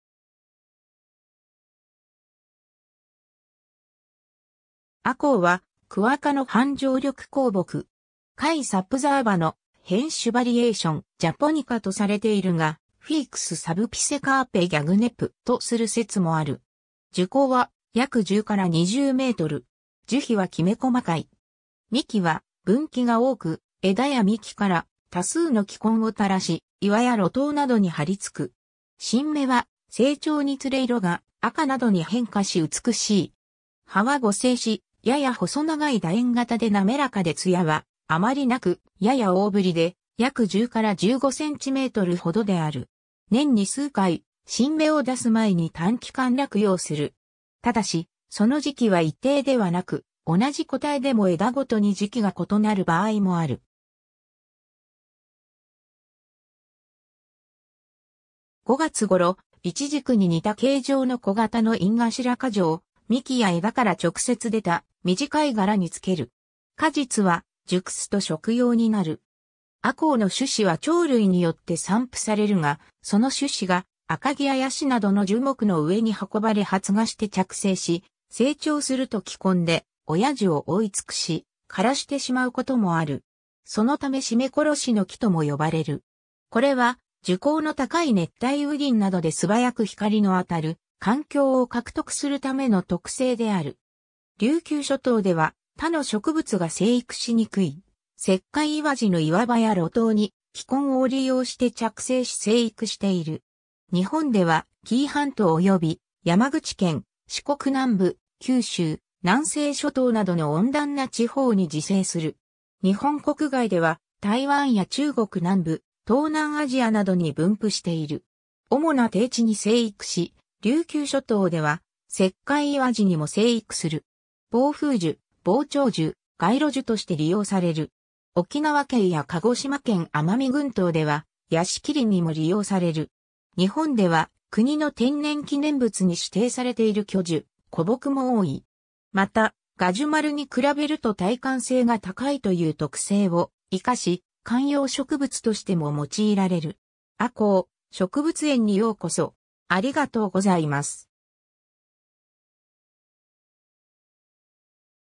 アコウ (植物)
アコウ（榕、赤榕、赤秀、雀榕、学名：Ficus superba var. japonica）は、クワ科の半常緑高木。
F. superba の変種 var. japonica とされているが、Ficus subpisocarpa Gagnep.とする説もある。
 樹高は約10 - 20m。
樹皮はきめ細かい。
幹は分岐が多く、枝や幹から多数の気根を垂らし、岩や露頭などに張り付く。
新芽は成長につれ色が赤などに変化し美しい。
葉は互生し、やや細長い楕円形でなめらかでつやはあまりなく、やや大ぶりで約10 - 15cm程である。
年に数回、新芽を出す前に短期間落葉する。
ただし、その時期は一定ではなく、同じ個体でも枝ごとに時期が異なる場合もある。
 5月頃、イチジクに似た形状の小型の隠頭花序を、幹や枝から直接出た短い柄に付ける（幹生花）。
果実は熟すと食用になる。
 アコウの種子は鳥類によって散布されるが、その種子がアカギやヤシなどの樹木の上に運ばれ発芽して着生し、成長すると気根で親樹を覆い尽くし、枯らしてしまうこともある。
そのため絞め殺しの木とも呼ばれる。
これは樹高の高い熱帯雨林などで素早く光の当たる環境（樹冠）を獲得するための特性である。
琉球諸島では、他の植物が生育しにくい石灰岩地の岩場や露頭に、気根を利用して着生し生育している。
 日本では、紀伊半島及び山口県、四国南部、九州、南西諸島などの温暖な地方に自生する。
日本国外では台湾や中国南部、東南アジアなどに分布している。
 主な低地に生育し、琉球諸島では石灰岩地にも生育する。
 防風樹、防潮樹、街路樹として利用される。
沖縄県や鹿児島県奄美群島では、屋敷林にも利用される。
日本では国の天然記念物に指定されている巨樹、古木も多い。
 また、ガジュマルに比べると耐寒性が高いという特性を活かし、観葉植物としても用いられる。
 アコウ:植物園にようこそ